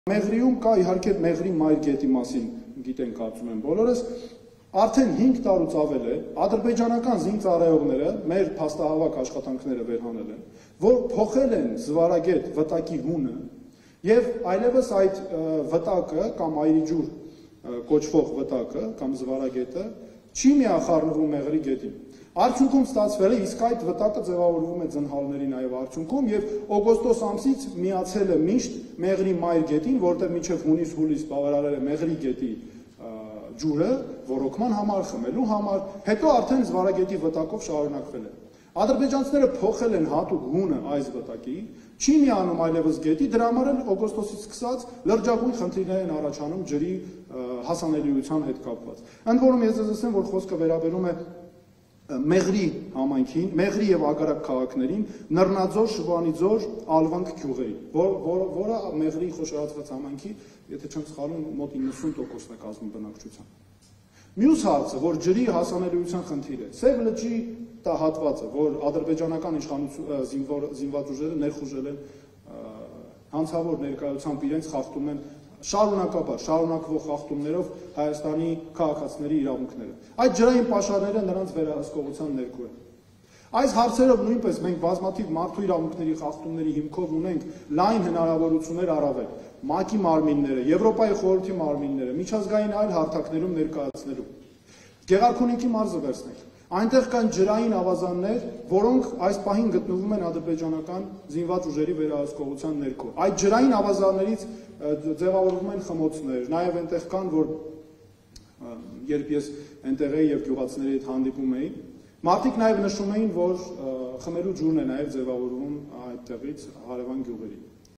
Ադրբեջանցիները փոխել են Զվարագետ վտակի հունը, որը չի միանում Մեղրի գետին. Արման Թաթոյան
Ադրբեջանցիները փոխել են Զվարագետ վտակի հունը, որը չի միանում Մեղրի գետին. այդ պատճառով ջրի հասանելիության լրջագույն խնդիրներ են առաջանում. ՄԻՊ